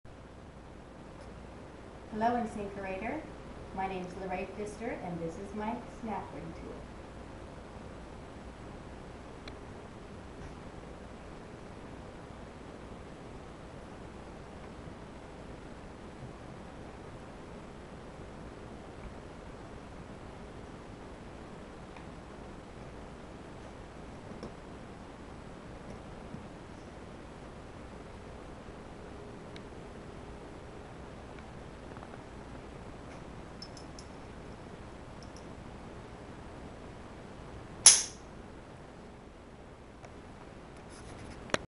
Lorae Duncan Snap Ring Tool
Lorae Duncan invented the Snap Ring Tool to make it easier for the plumber and homeowner to install the Snap Ring onto the garbage disposal.